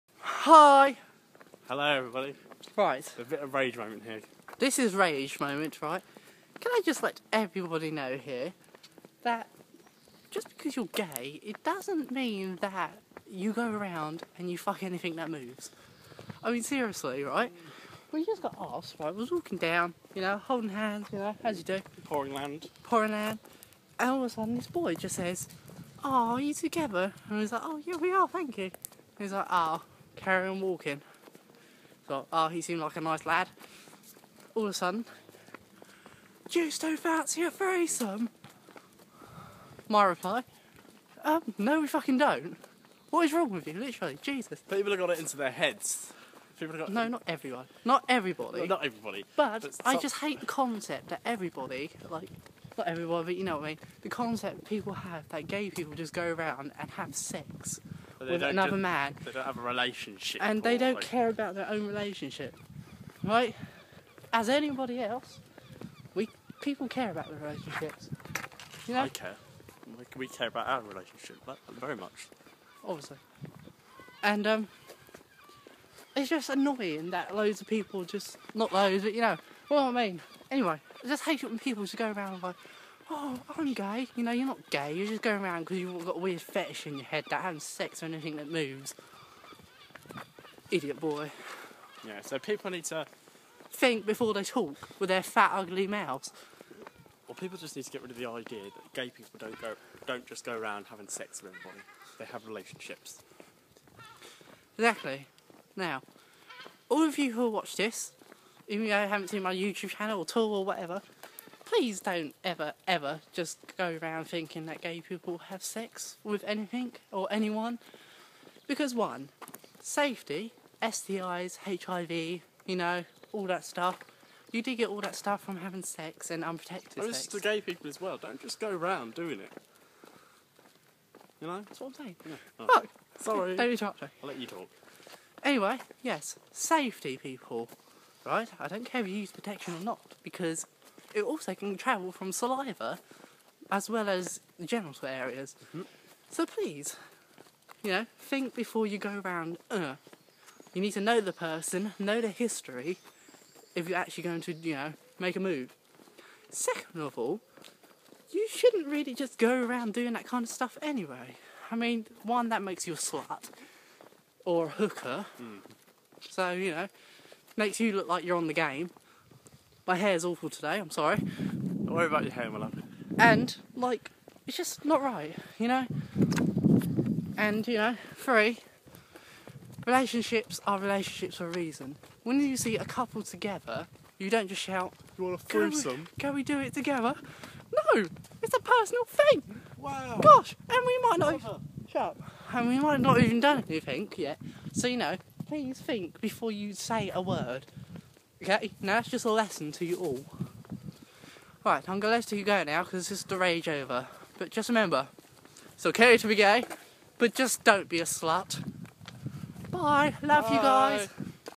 It's okay to be gay not a slut!
So me and Dan were walking around Poringland and this guy asked if we were together and I said yes etc etc, anyway as we walk away he asked if we wanted a three some... What the hell!?!?!?
We're a couple for a reason, just because we bloody gay it doesn't mean we go around and shag anything that moves!!!
Anyway this is my rage video.

LoveLove! Xx